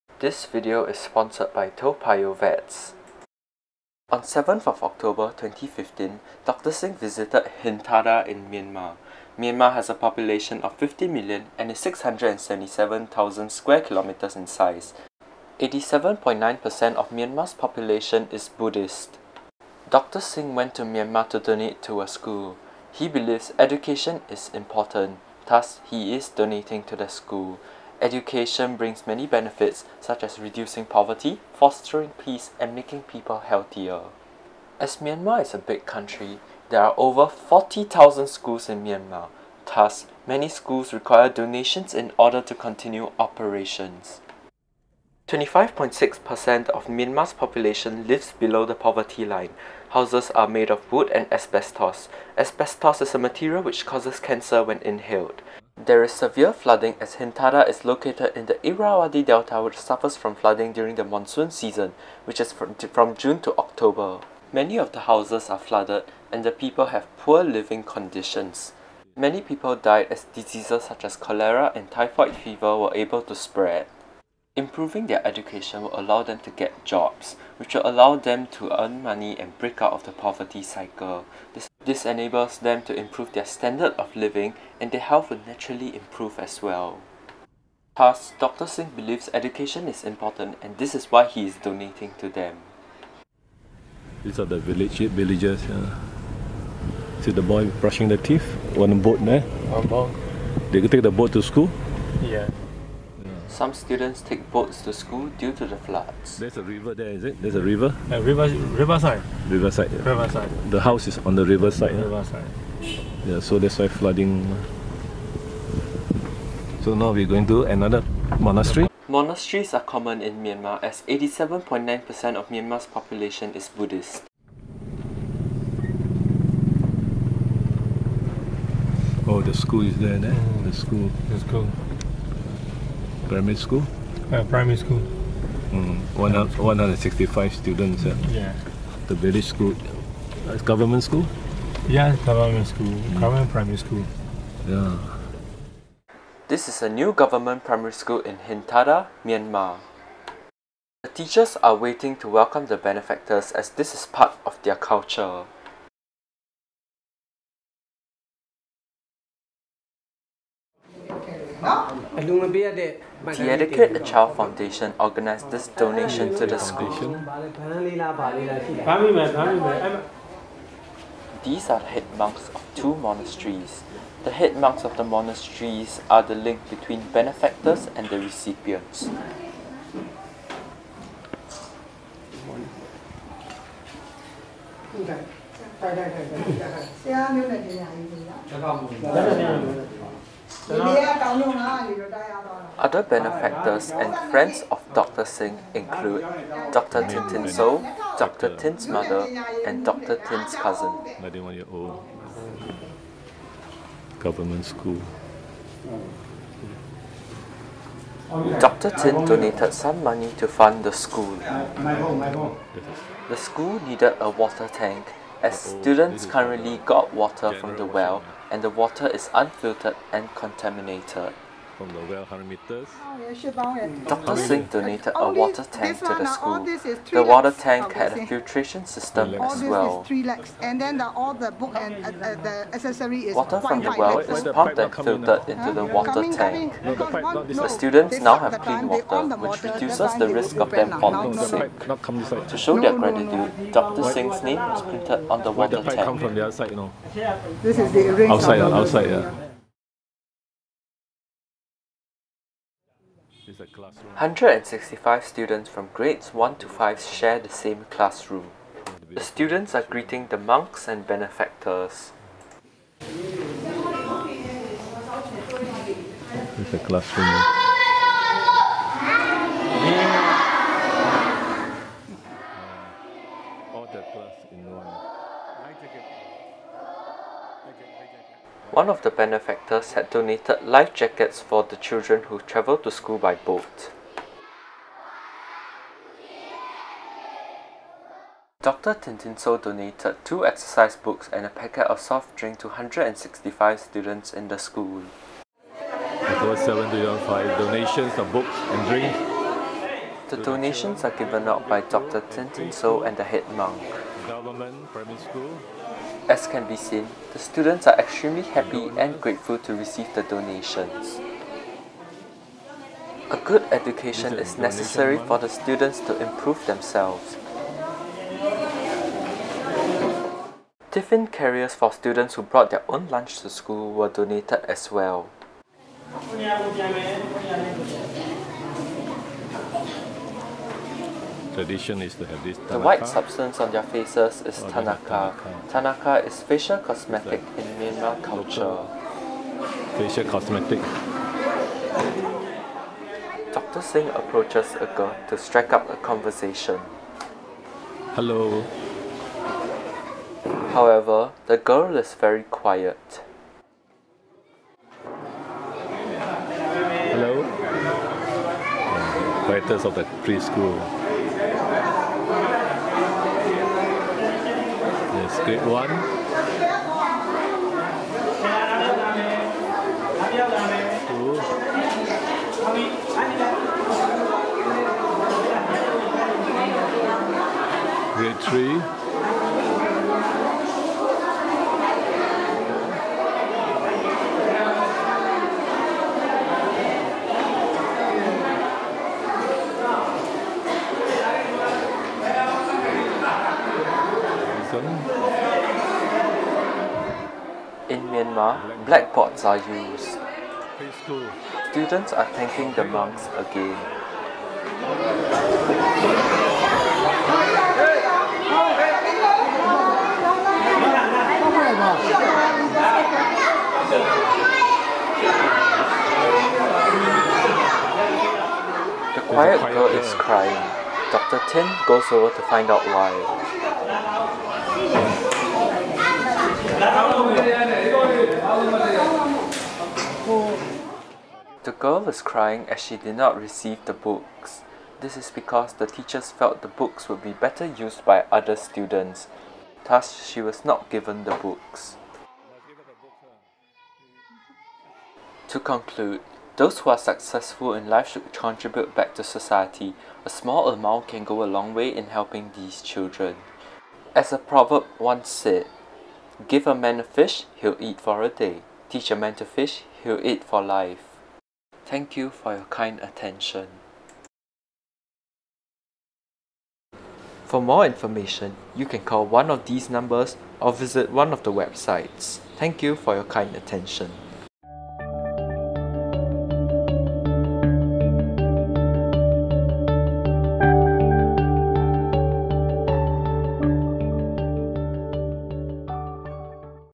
MYANMAR TRAVEL: A stainless steel water tank to provide filtered water for the students
In Oct 7, 2015, Dr Sing Kong Yuen of Toa Payoh Vets visited Hinthada on the invitation of local veterinarians, Drs Thin Thin Soe and Dr Thein Tun Aung.

Dr Thin Thin Soe's grandfather and father has maintained a long tradition of celebrating the Thadingyut Festival in October 2015 by donations of alms to over 1,000 monks in Hinthada, Myanmar.

Dr Sing Kong Yuen gave a SGD 300 to donate exercise books and pencils to a government primary school. So Dr Thin Thin Soe arranged for this donation to be executed with the Head Monk of her monastery. The school principal requested for a water tank instead and much appreciated the donation by acknowledging Dr Sing's name on the water tank.  Apparently, it is the custom to record the name of donors whenever practical.

This video is made to inspire younger successful Singaporeans to donate to improving the education in Myanmar's primary schools, within their financial means.

Many village schools need money for their writing materials as the parents are very poor.

This video shows Dr Sing Kong Yuen donating a small sum  of SGD 300 to buying writing books and pencils to the school . However, the school said it needed a stainless steel water tank to provide clean filtered water to the students instead of exercise books. Dr Sing's friend, Dr Thin Thin Soe donated the exercise books and a drink to all the 165 students. However, one small quiet girl did not receive the books and drink. The teacher felt that the exercise books are in such short supply and since the quiet girl would not be using the book. She was given the presents as well.

For those younger Singaporeans who succeed in life, donations to education of Myanmar needing help need not be in large amounts as shown in this video. Small amounts of $100 or less are much appreciated and make a difference to the future of village school students and ultimately to the younger generation of Myanmar people.

Readers interested in donating to buying exercise books, pencils and sharpeners to village primary school students, please email or tel/sms Dr Sing Kong Yuen at . Donations can be as little as $50.00 and will be accounted for.

The head monks facilitates the donations from the benefactors to the students as shown in this video.